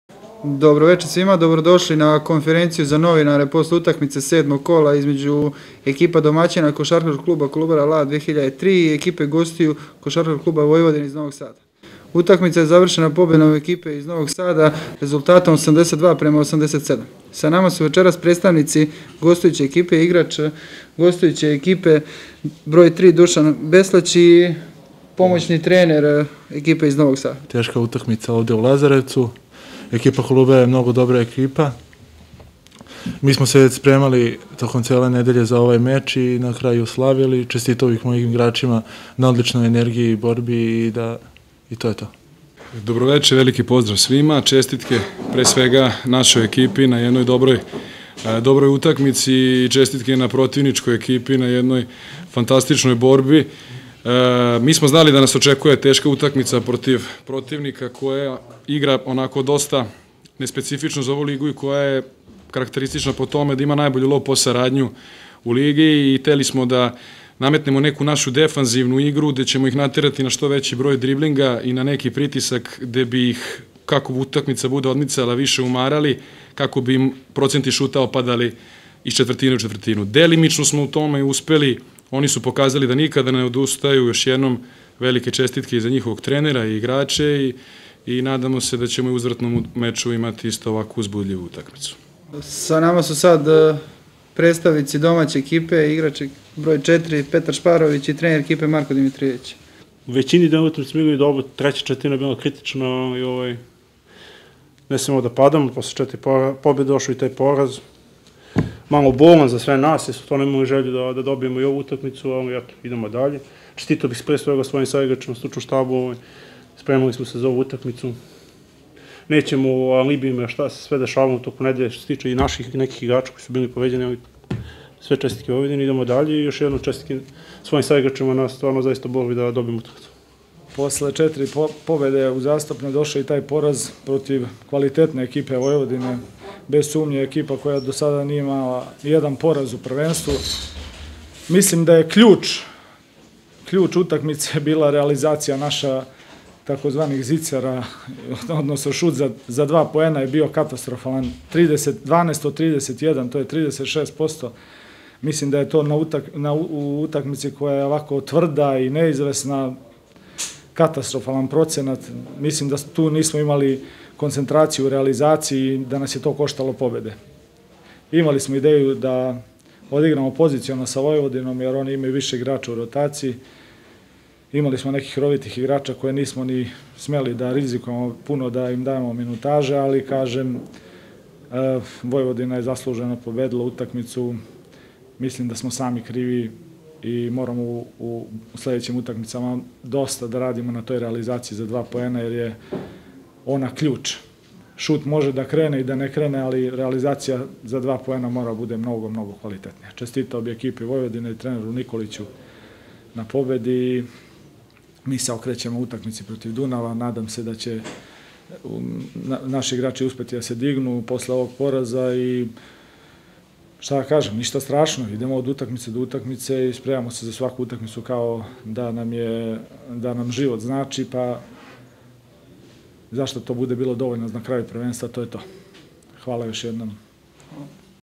Press konferencija KK Kolubara KK Vojvodina (82:87) 7.kolo KLS 2020/21
Utisci iz hale Kolubara Lazarevac nakon odigranog susreta KK Kolubara KK Vojvodina (82:87) 7.kolo KLS 2020/21 Izjave igrača i trenera i pomoćnog trenera obe ekipe: Dušan Beslać i pom.trener Miloš Isakov Kovačević za Vojvodinu i Petar Šparović i trener Marko Dimitrijević za Kolubaru